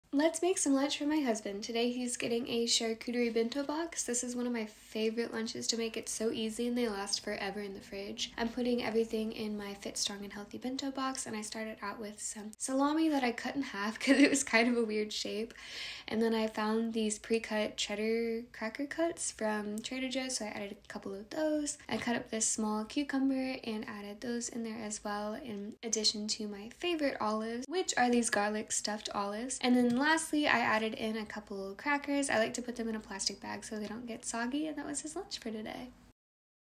Charcuterie Bento Box
I love making these charcuterie bento boxes for lunch because they are so simple, don’t have to be heated up, and they last a really long time in the fridge!!

My favorite hack lately has been to put the crackers inside a ziplock bag so they don’t get stale or soggy.